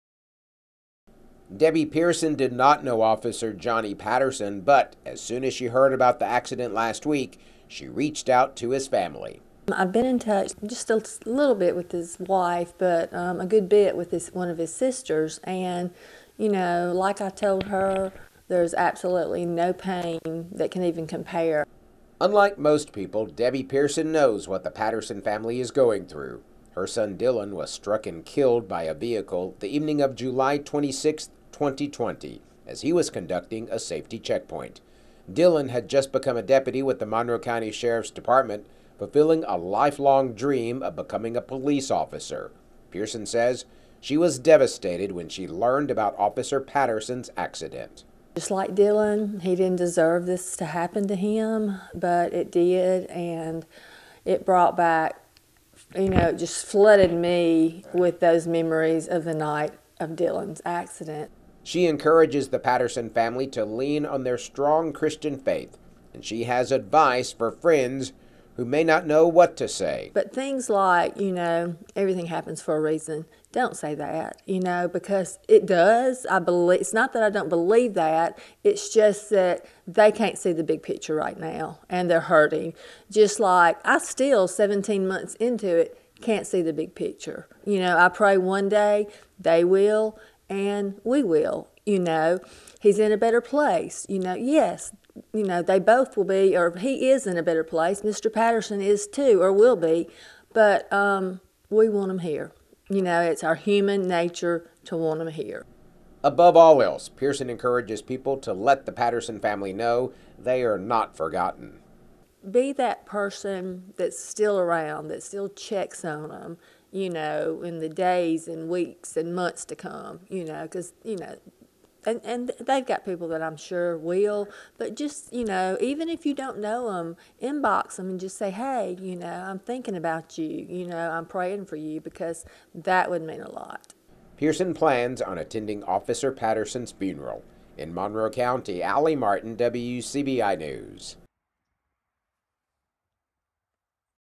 F DYLANS MOM REAX PKG 012122
The mother of a sheriff's deputy who was struck and killed while conducting a safety checkpoint shares her thoughts about a similar incident involving a Lee County School Resource Officer.

Officer Johnny Patterson was struck by a vehicle as he was directing traffic in front of Shannon Primary School.   His family made the tough decision to remove him from life support one week after the accident.